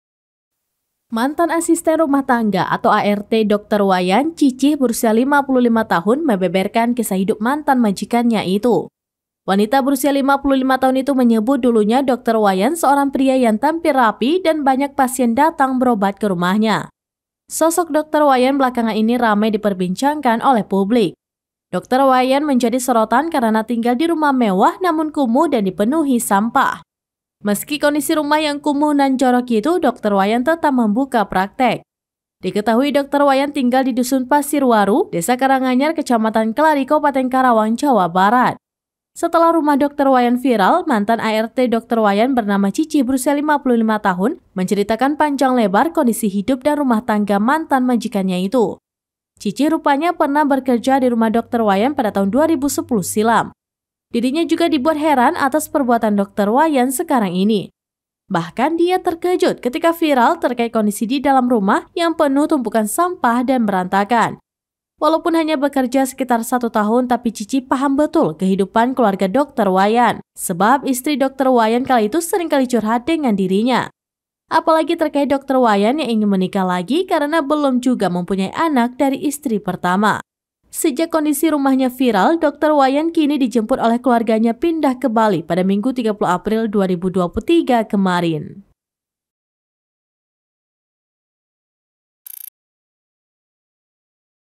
Mantan ART Ungkap Kehidupan Dokter Wayan Sebelum Ditinggal Istri Kedua
SERAMBINEWS.COM – Mantan Asisten Rumah Tangga (ART) Dokter Wayan, Cicih (55), membeberkan kisah hidup mantan majikannya itu.

Wanita berusia 55 tahun itu menyebut, dulunya dokter Wayan seorang pria yang tampil rapi dan banyak pasien datang berobat ke rumahnya.

Sosok dokter Wayan belakangan ini ramai diperbincangkan oleh publik.

Dokter Wayan menjadi sorotan karena tinggal di rumah mewah namun kumuh dan dipenuhi sampai.

VO: Syita
EV: T. Nasharul